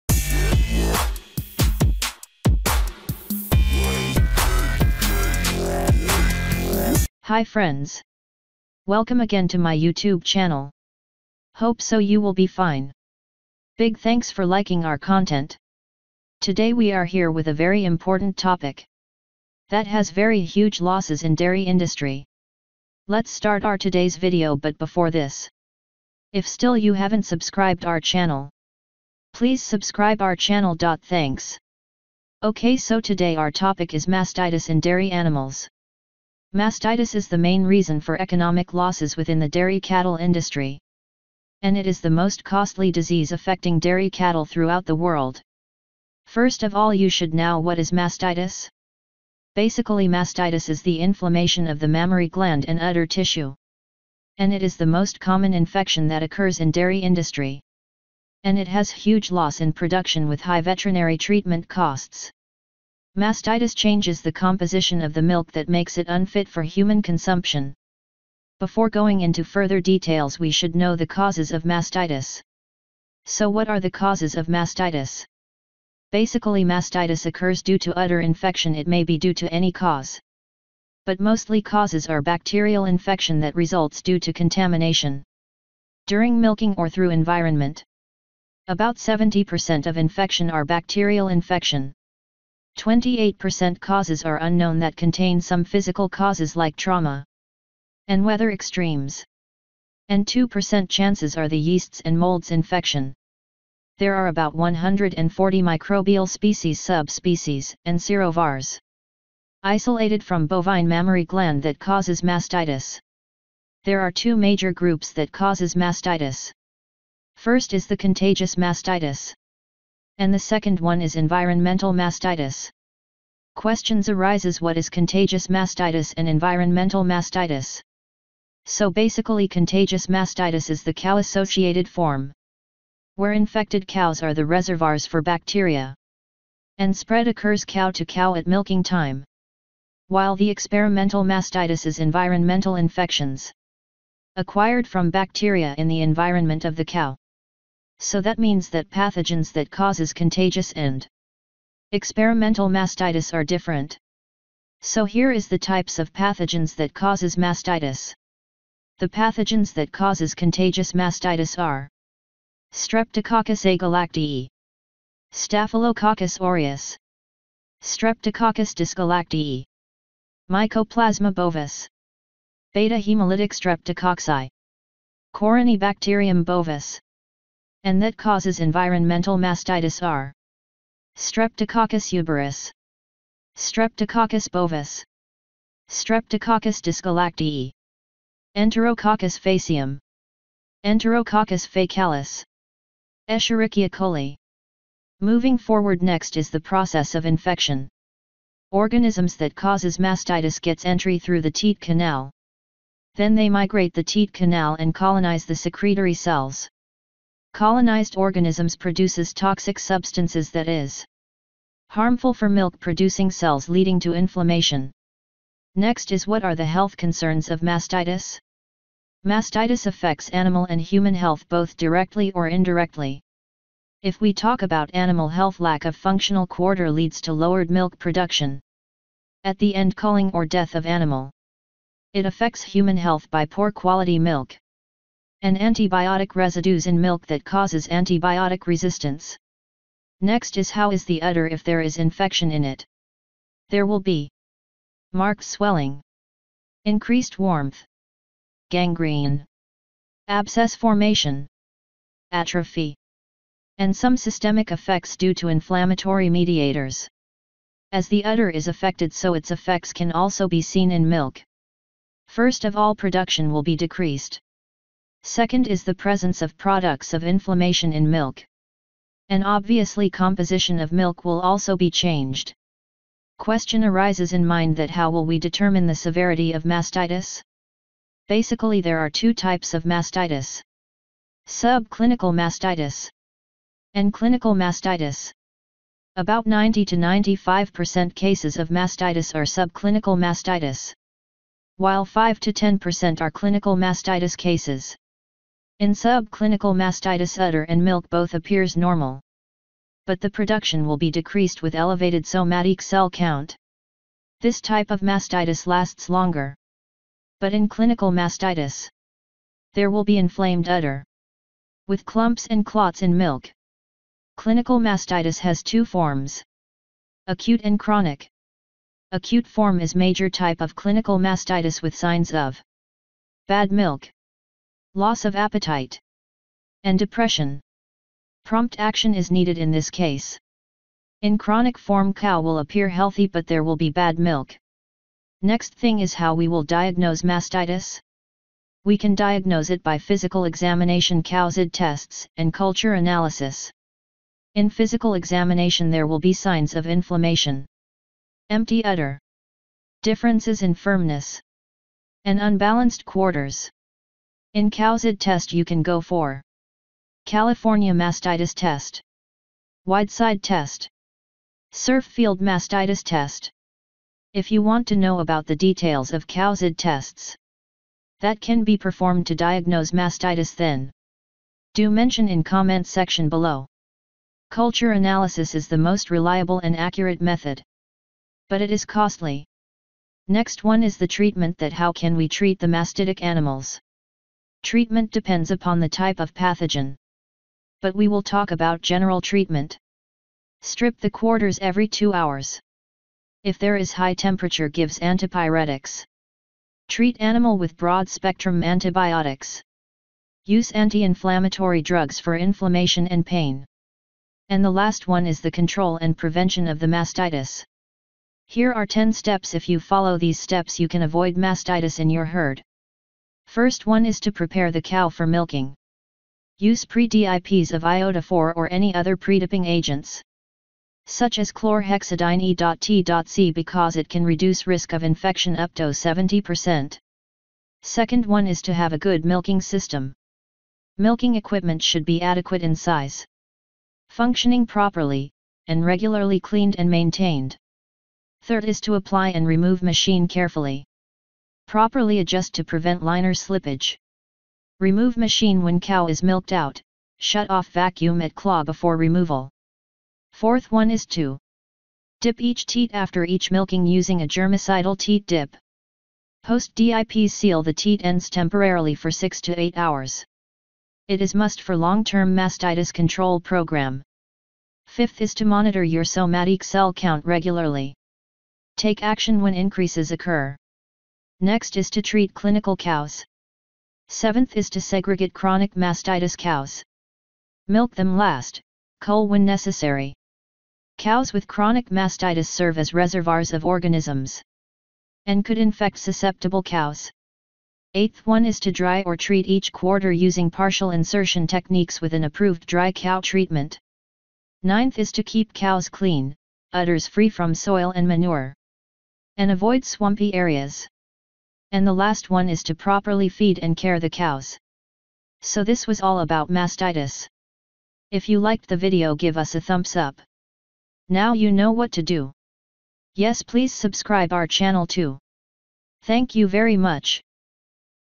Bovine mastitis II How to Identify mastitis II Acute and Clinical Mastitis II Treatment
Mastitis can be one of the most costly diseases in a dairy sector.

Mastitis is the inflammation of the mammary gland and udder tissue, and is  most common disease of dairy cattle. It usually occurs as an immune response to bacterial invasion of the teat canal by variety of bacteria present on the farm.

Milk-secreting tissues and various ducts throughout the udder can be damaged.
Mastitis is most often transmitted by contact with the milking machine, and through contaminated hands or other materials, in housing, bedding and other equipment.

Mastitis changes the composition of the milk that makes it  unfit for human consumption.
About 70 percent of infection are bacterial infection
28 percent causes are unknown that contain some physical causes like trauma and weather extremes.
And 2% chances are the yeasts and molds infection.
There are about  140 microbial species ,sub species and serovars
 isolated from bovine mammary gland that causes mastitis.